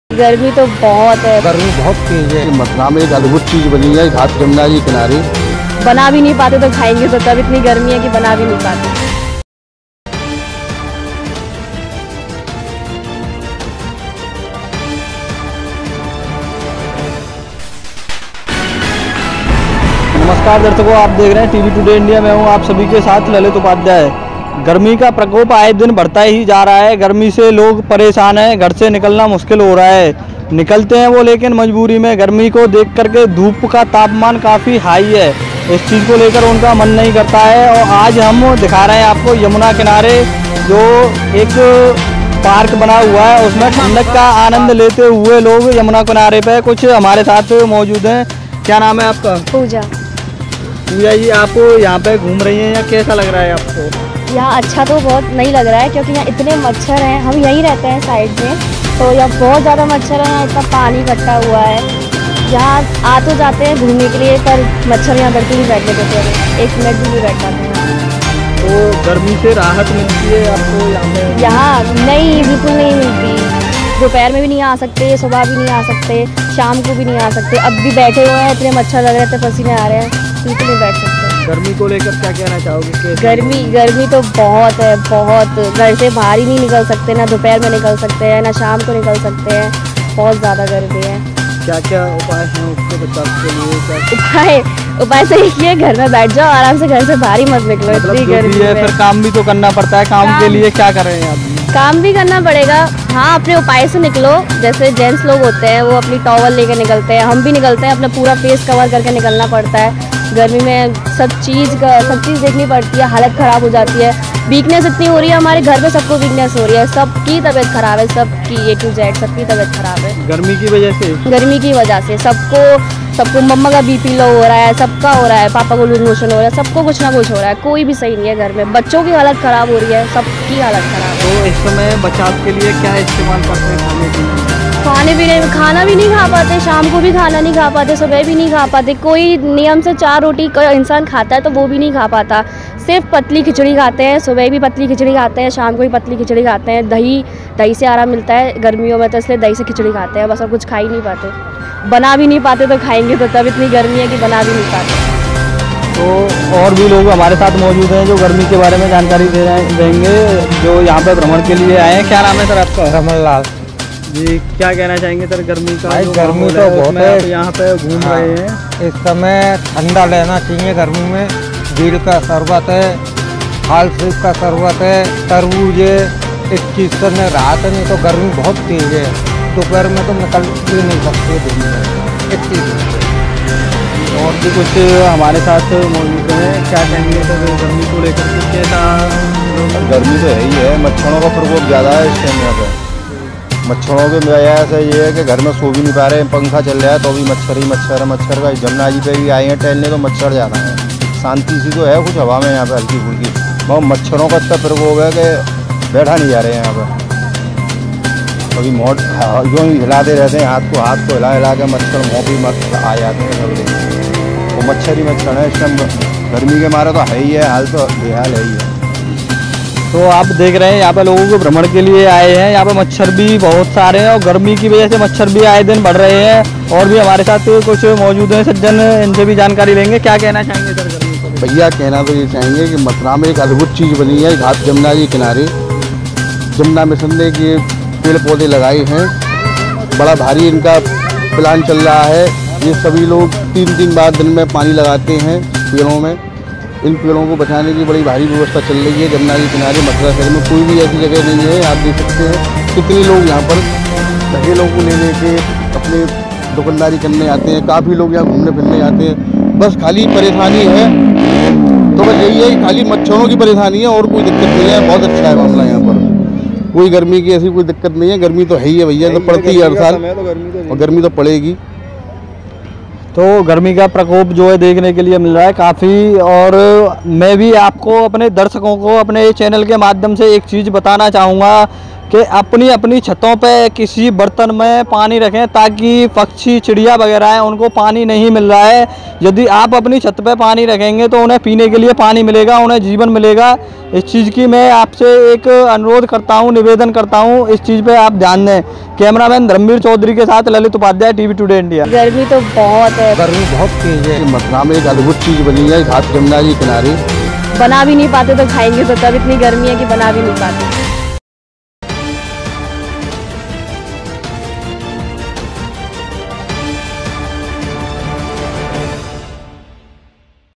TTI Mathura Ka Mausam | लोगों का हाल बेहाल | यमुना किनारे चैन नहीं, मच्छरों का प्रकोप, क्या बोले लोग
#@mathuranews mathuralive wheathernews TTI Mathura Ka Mausam | लोगों का हाल बेहाल | यमुना किनारे चैन नहीं, मच्छरों का प्रकोप, क्या बोले लोग